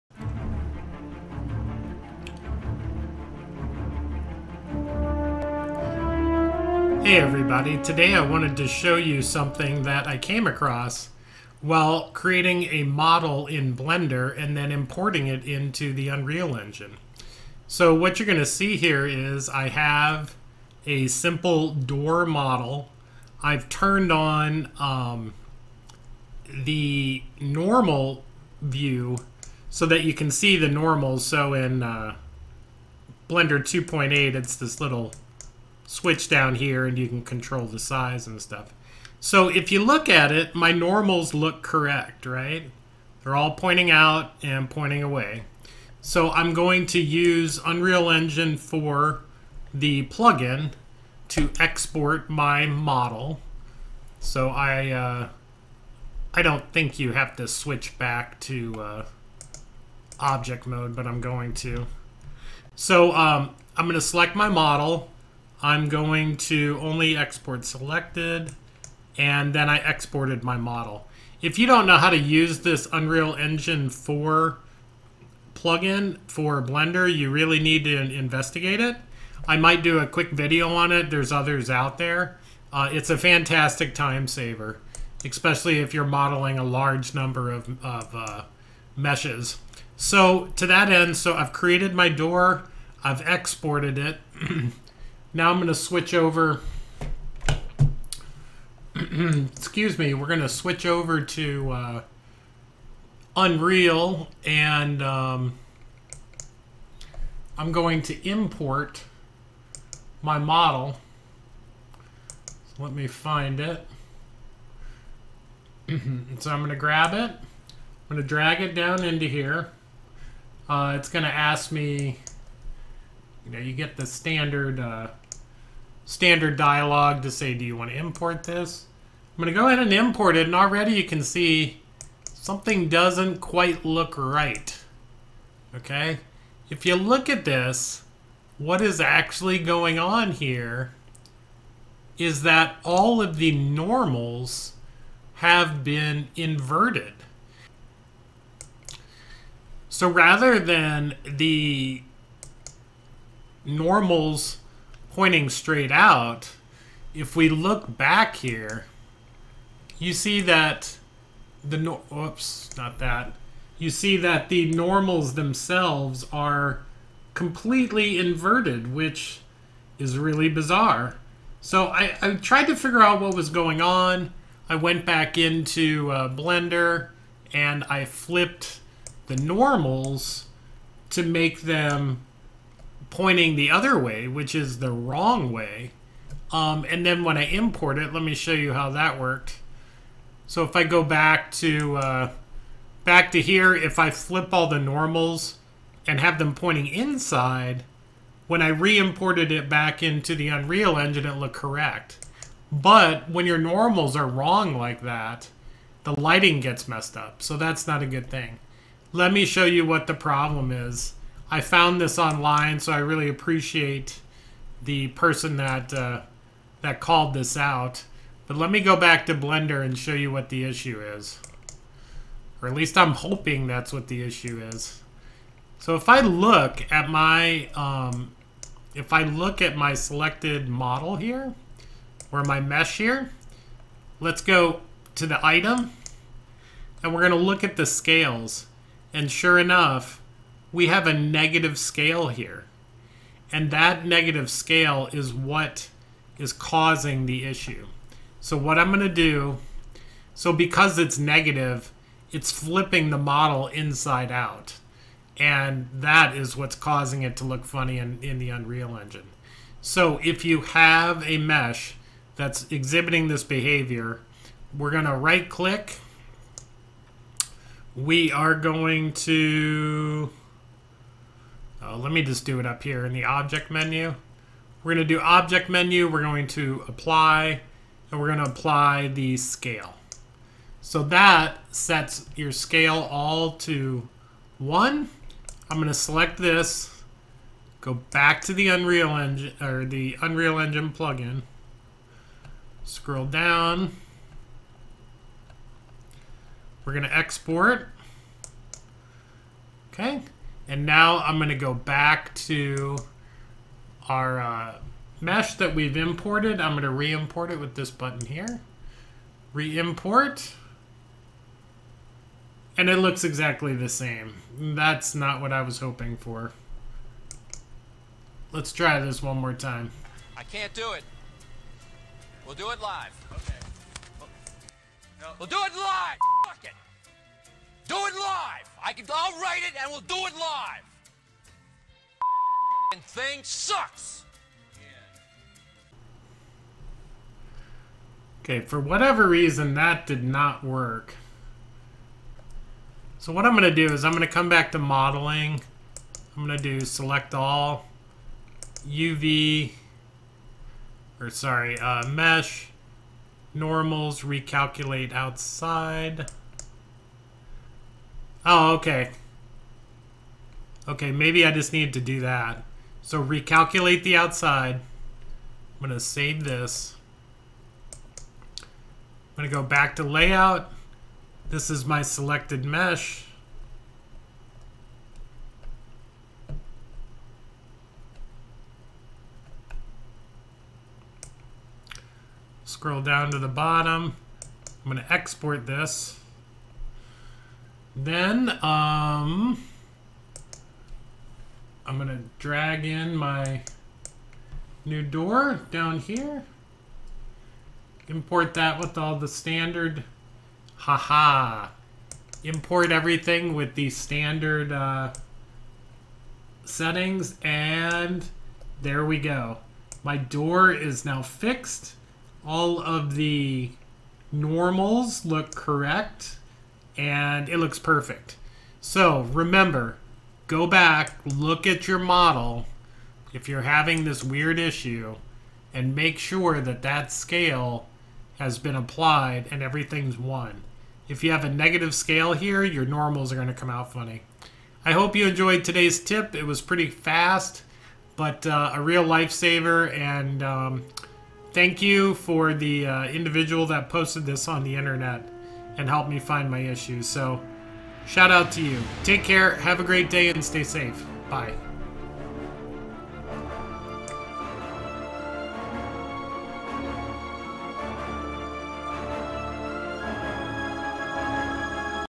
Flipped/Inverted Normals Fix (Blender to UE4)
Fixing inverted normals on a mesh created in blender and imported into the Unreal Engine.

I am a new channel and could use all the help I can  get, thanks!